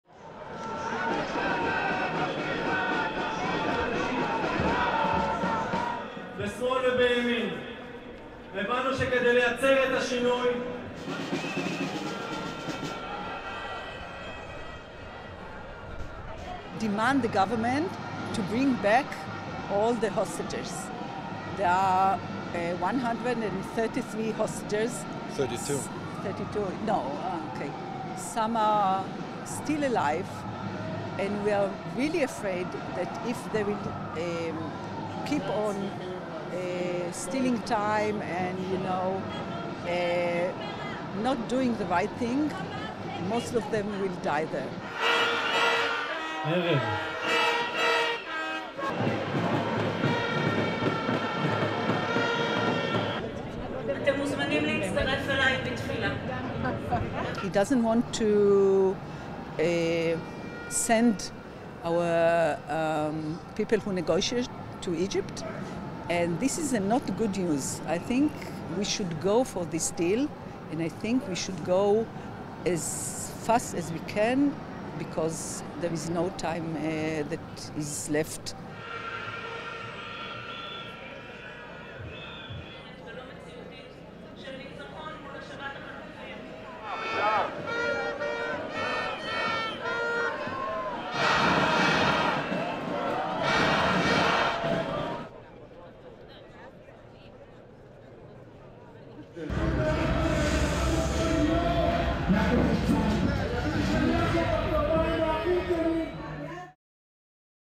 Thousands of Israelis attend anti-government rally in Tel Aviv | AFP
Thousands of Israelis protest in Tel Aviv against the government of Prime Minister Benjamin Netanyahu, urging the veteran right-wing leader to negotiate with Hamas to bring back the hostages held by the Palestinian militant group in the Gaza Strip. Hamas has accused Netanyahu of sabotaging truce talks in Cairo, the latest bid to halt the war in Gaza which has raged for more than half a year and devastated the coastal Palestinian territory.